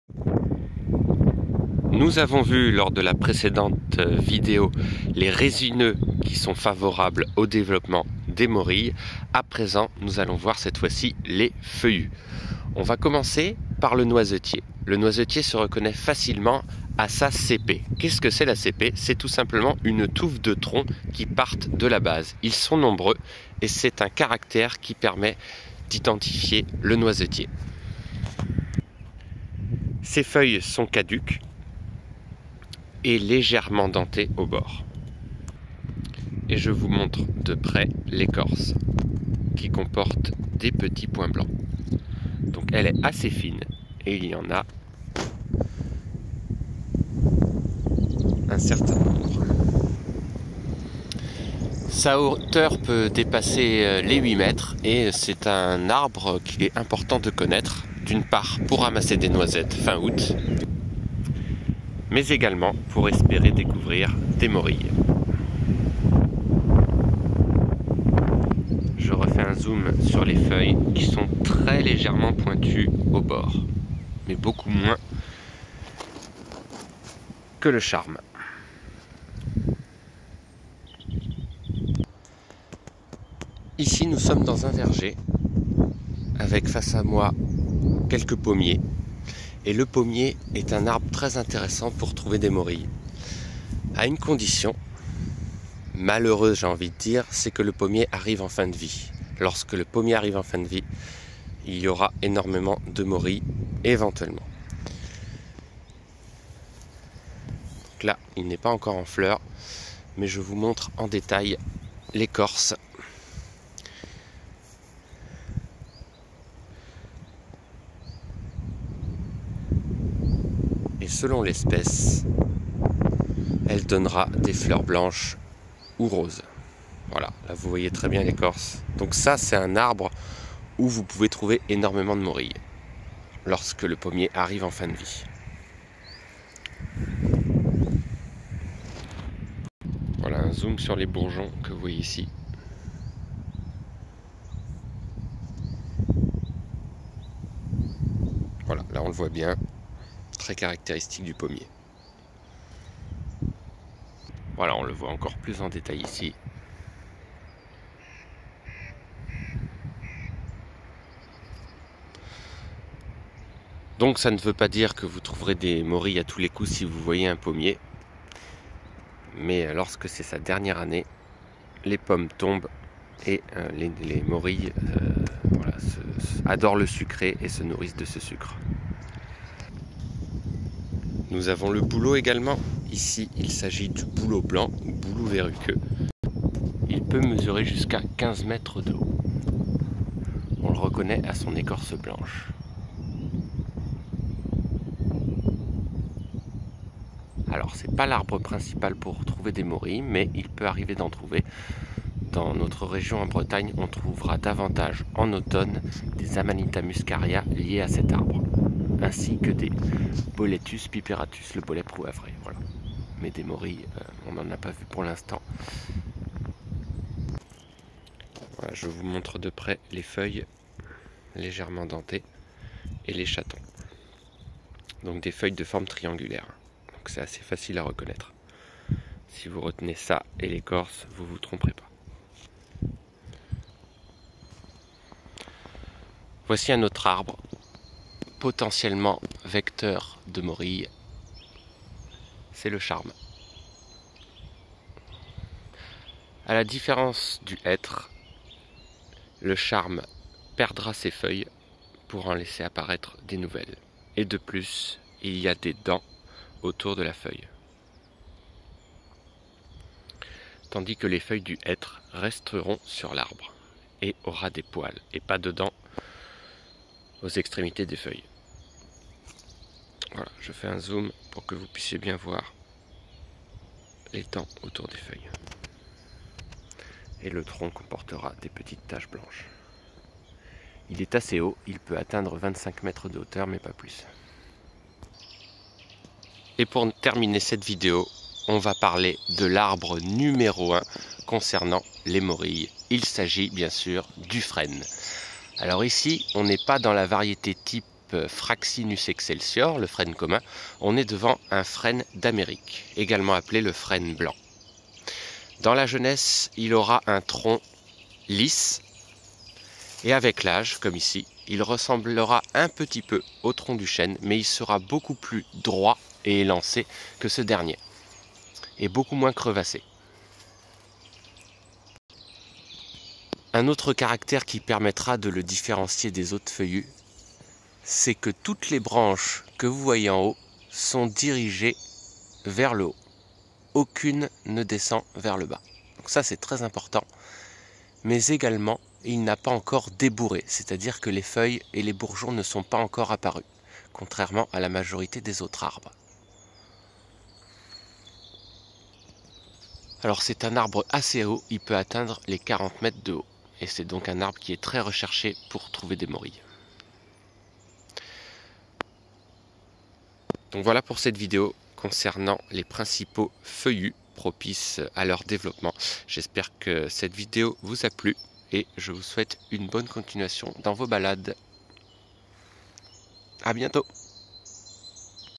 Morilles : identifier les arbres 🌳 (feuillus)
Dans cette vidéo, vous trouverez les arbres feuillus les plus favorables pour découvrir des Morilles. Bon visionnage 😎